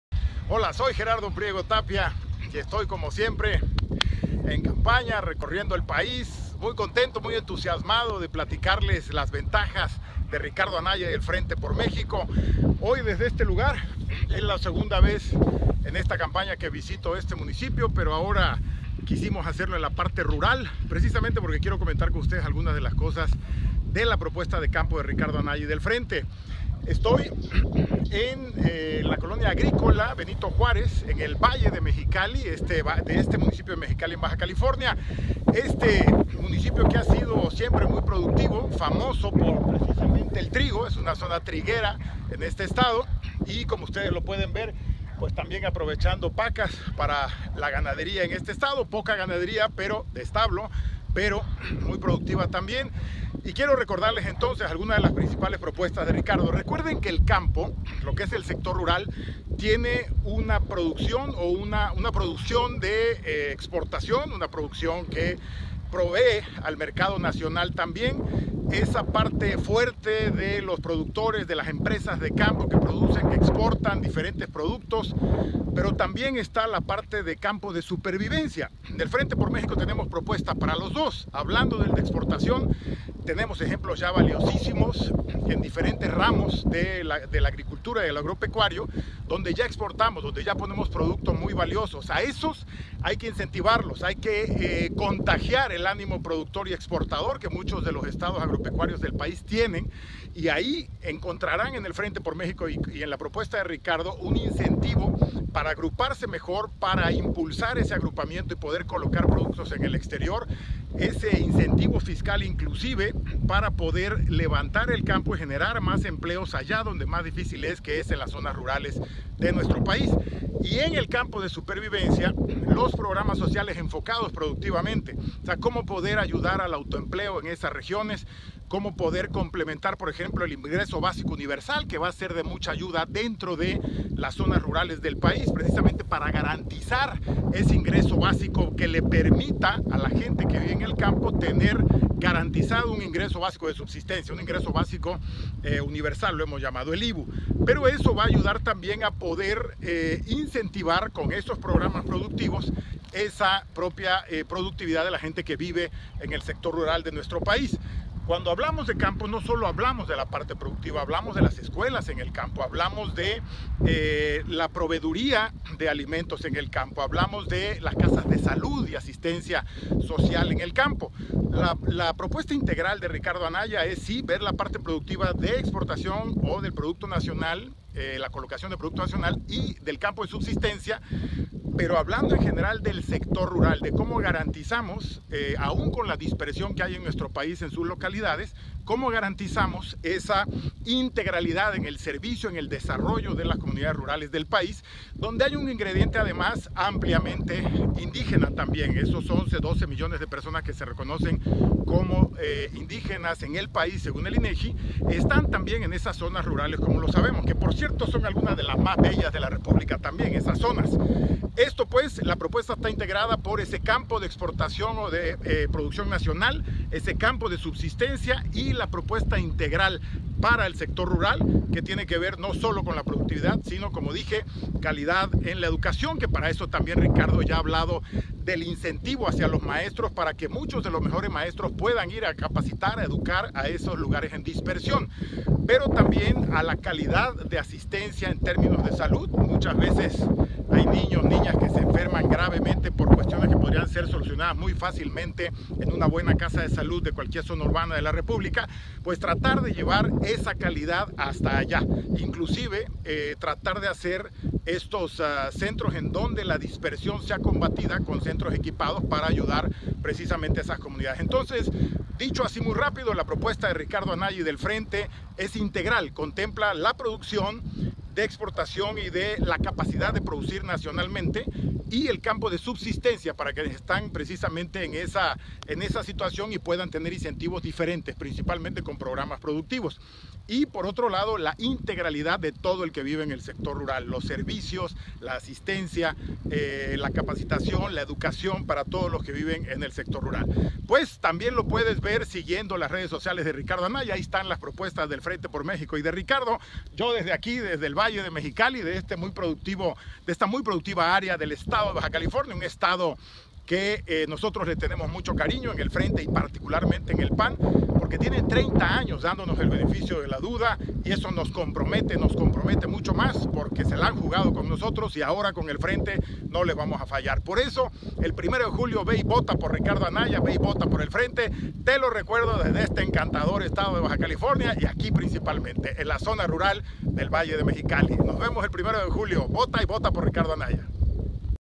Gerardo desde Baja California en la Agricola Juarez,nos comenta propuestas para el México Rural
Gerardo desde la Colonia Agricola Benito Juárez en el Valle de Mexicali, Baja California, zona productora de Trigo por excelencia, nos explica ángulos y enfoques de la propuesta para el México Rural de @RicardoAnayaC  .